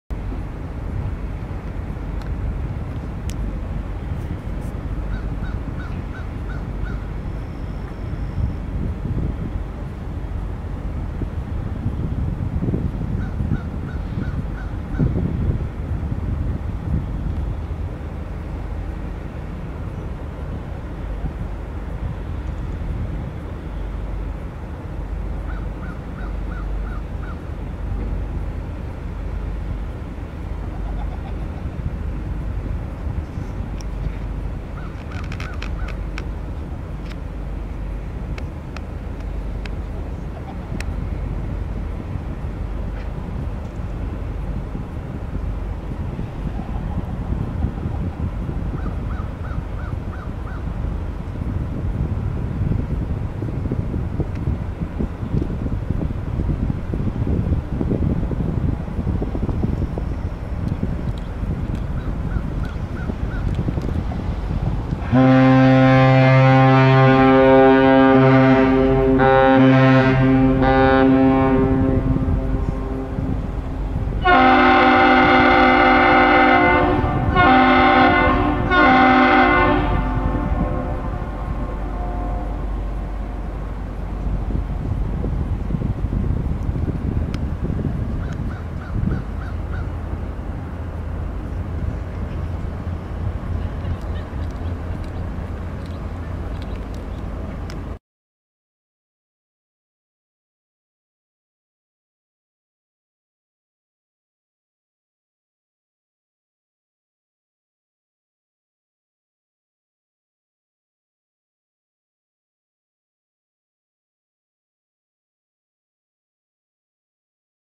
Walter J McCarthy Dep 9617
Walter J McCarthy Jr nicely departing during the lunch time for me this Wednesday noon, loaded with coal from Midwest Energy. Nice Captain's salute for the afternoon ship revelers...